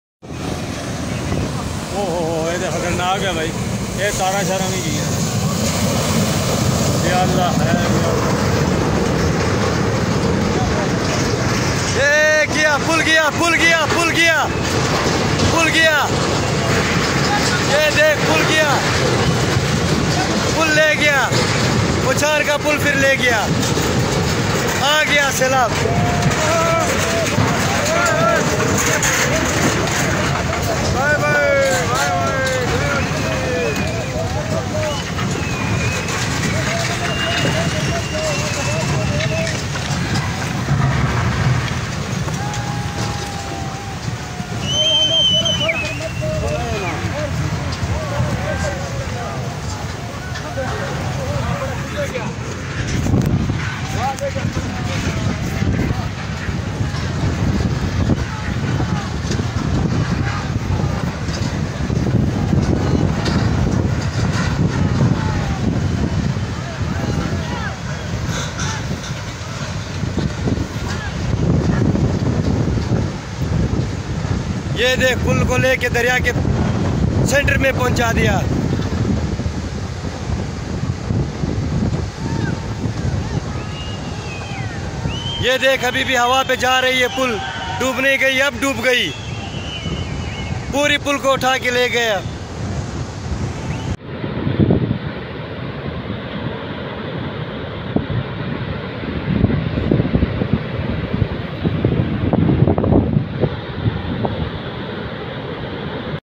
Flood in pakistan | bridge collapse at uchar nala kohistan ..
Himalaya productions